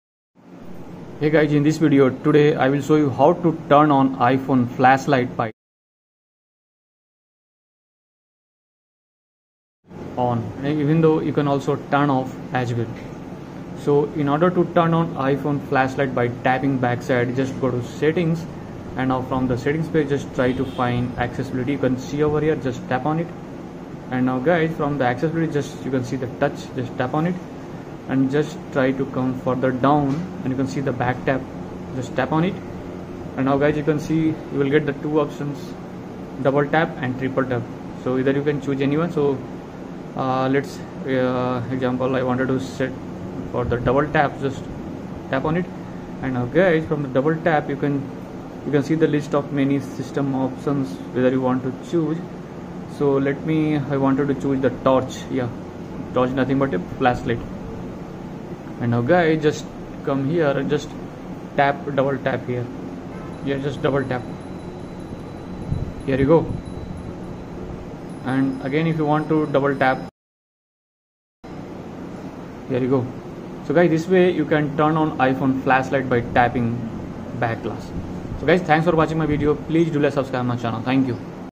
How to turn on iPhone flashlight by tapping
How to turn on iPhone flashlight by tapping - How to turn on flashlight by tapping back of iPhone || iOS 18 Back tap iPhone

Learn in this iPhone tips and tricks video, I show you how to turn on or off your iPhone flashlight by tapping the back glass.